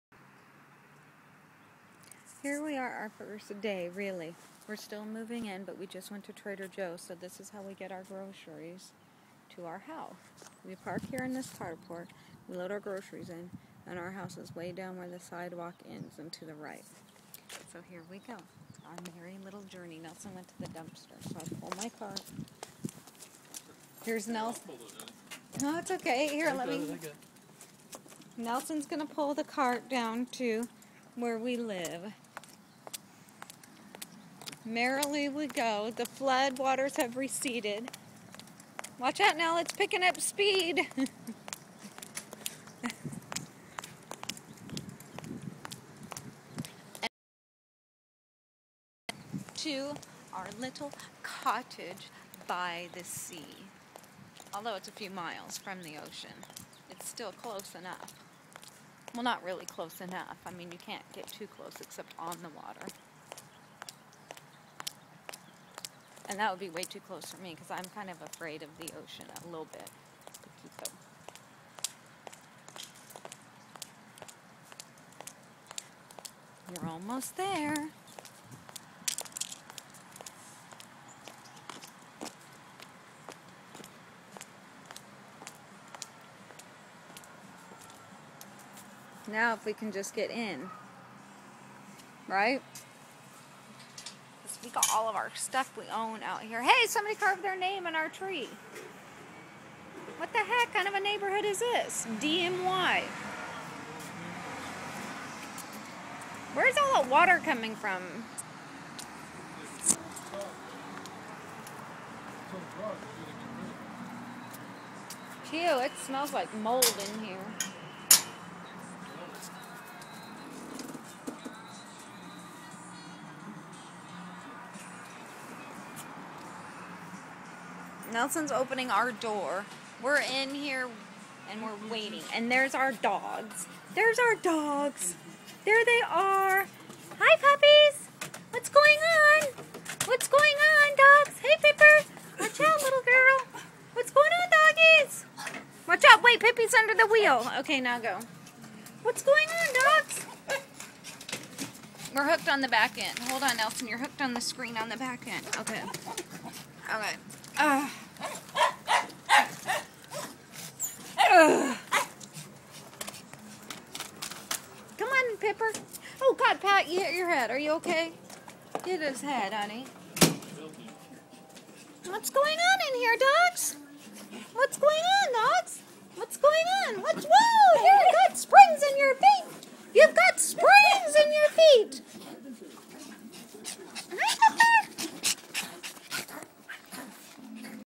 Trina Foster In...OUR Very 1st Morning Moved BACK IN to our little cottage
Our 1st day moved BACK in to our rental flat since the floods..